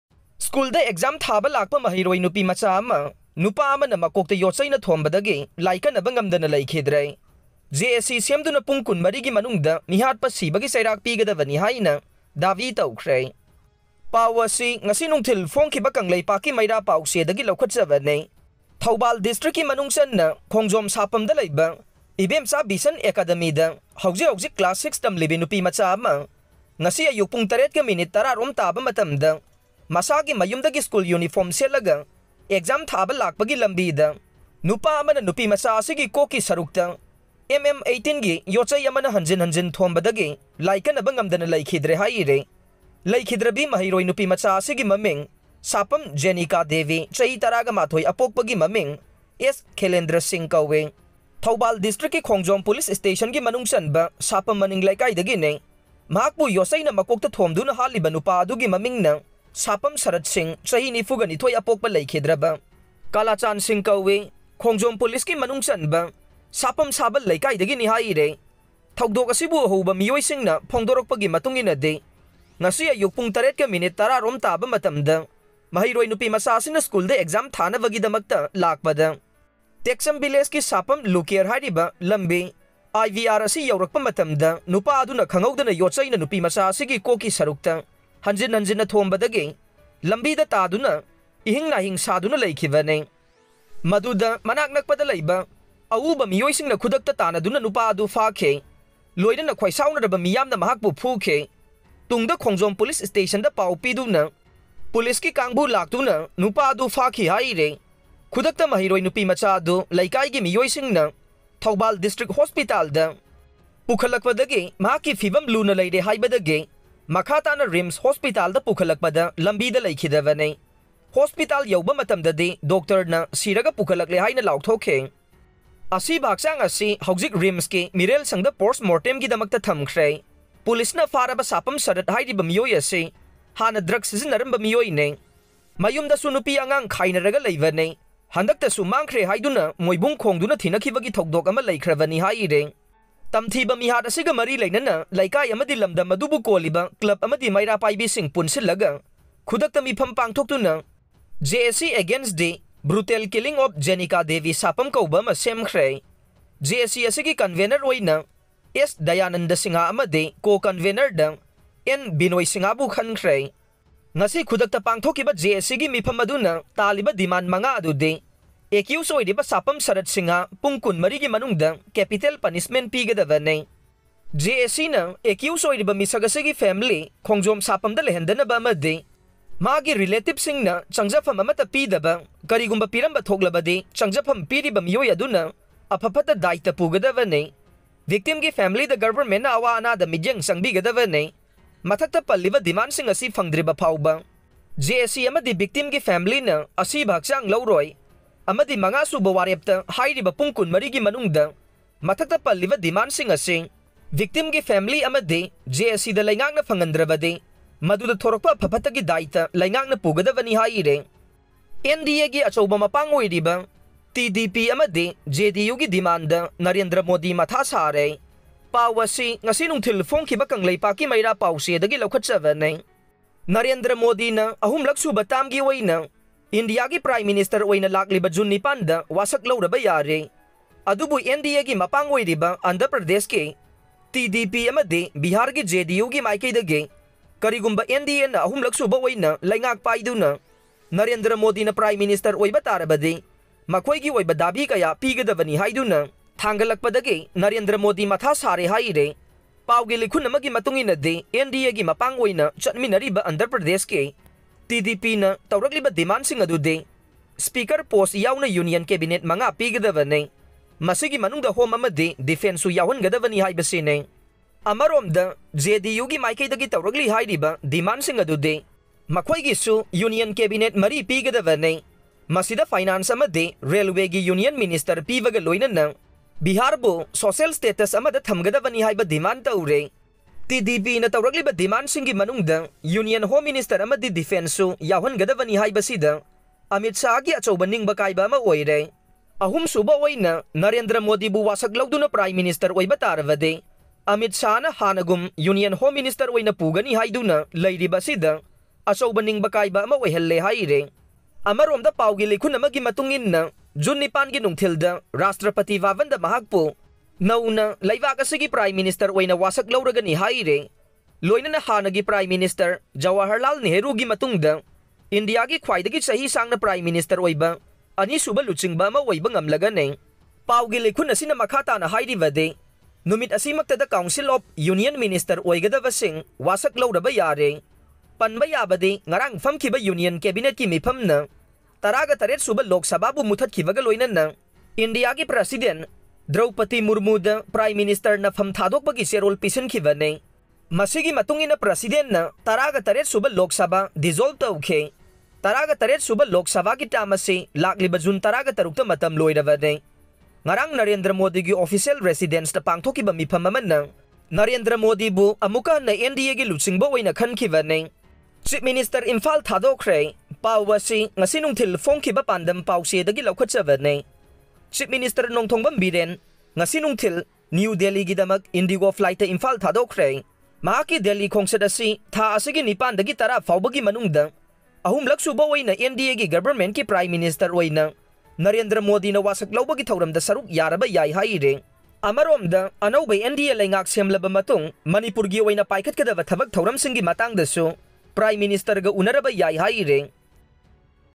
TDP Amadi JD(U) gi Demand da Narendra Modi Matha Sare 👉CM Imphal Thadokhre
School Exam Thaba Lakpa Maheiroi Nupimacha 1 Nupa 1 na Makokta Yotcheina Thombadagi Laikannaba Ngamdana Leikhidre

TDP Amadi JD(U) gi Demand da Narendra Modi Matha Sare

CM Imphal Thadokhre